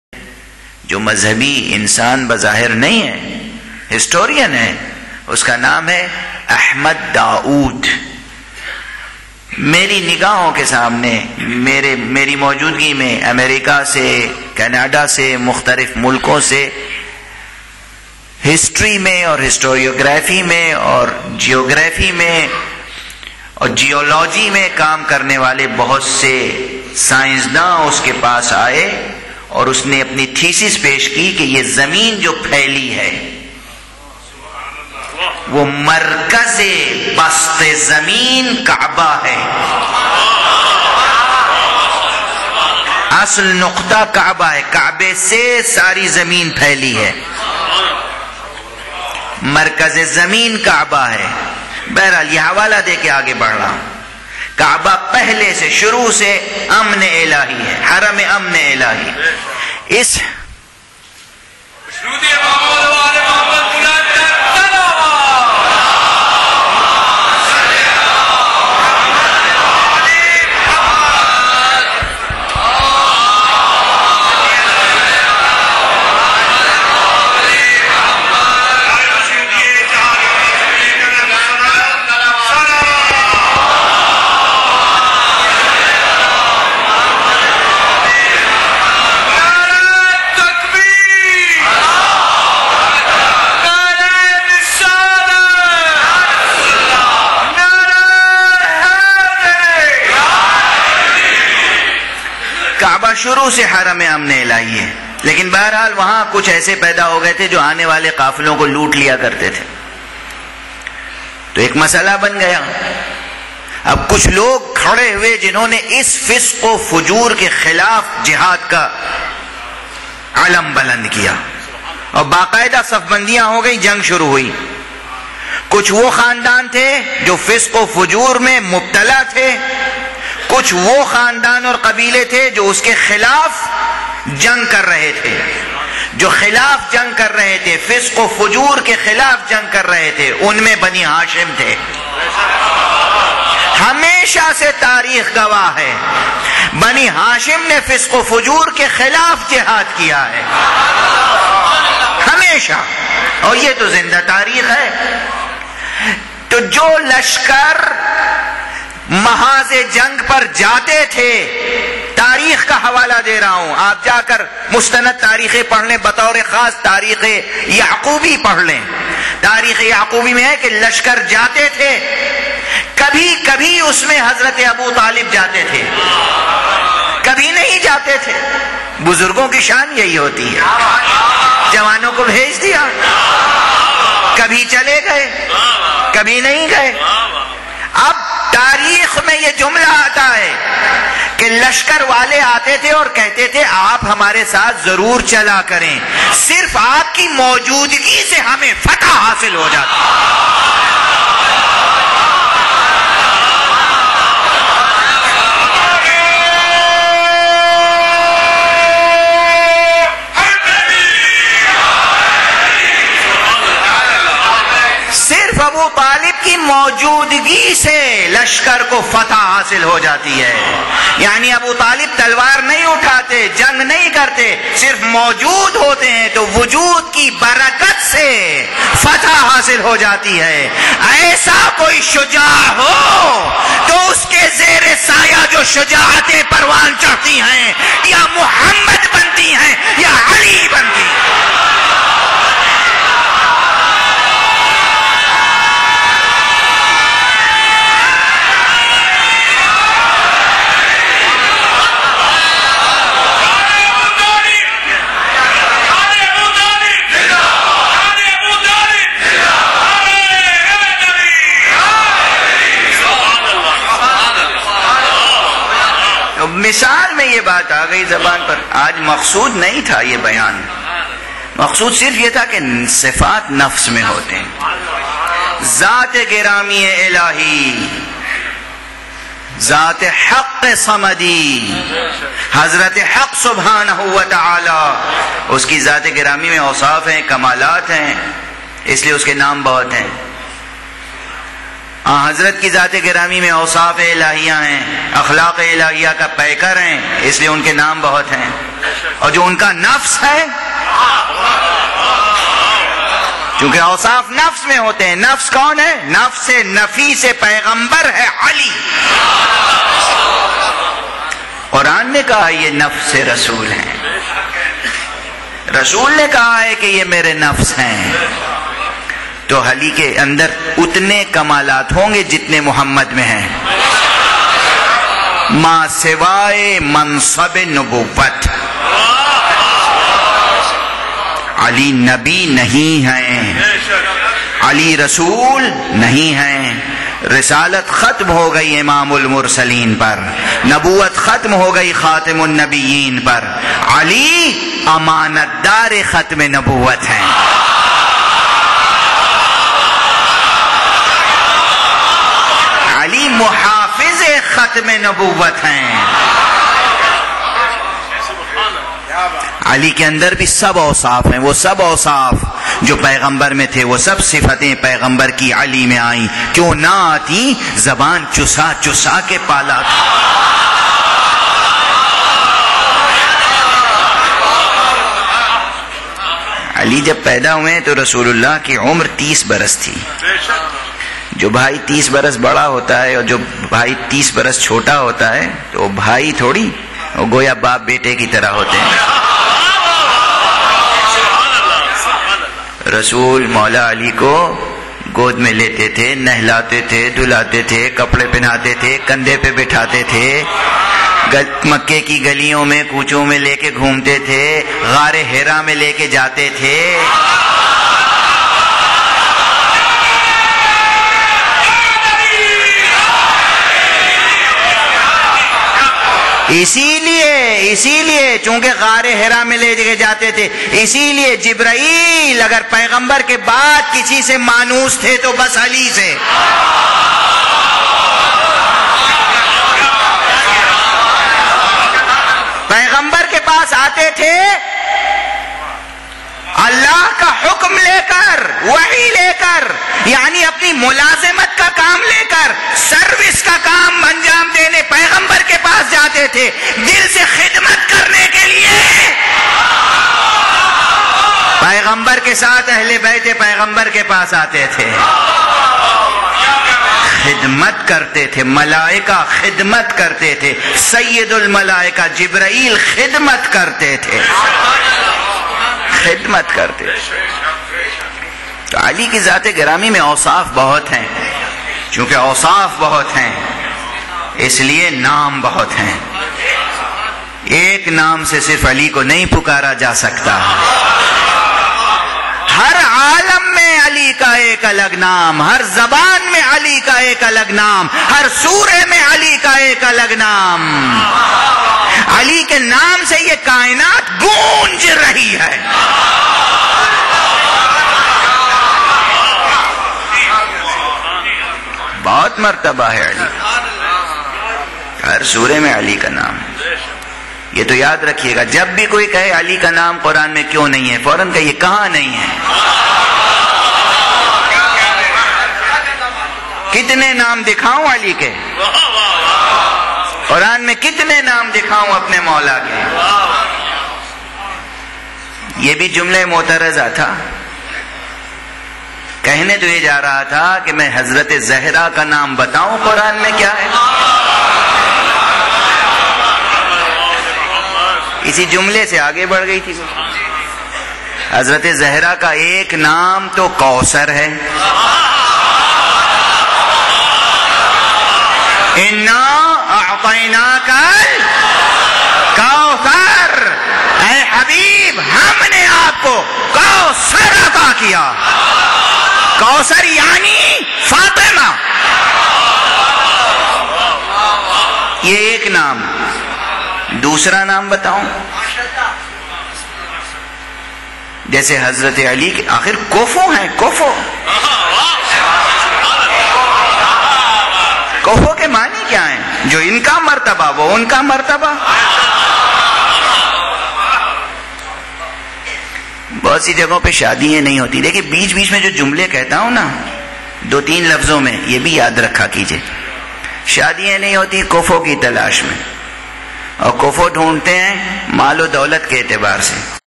Majlis - Ahl e Zikr Part F - Ayatollah Syed Aqeel Gharavi
Addressed at Jamia Masjid, Haider Road, Krishan Nagar, Lahore on 20-Jan-10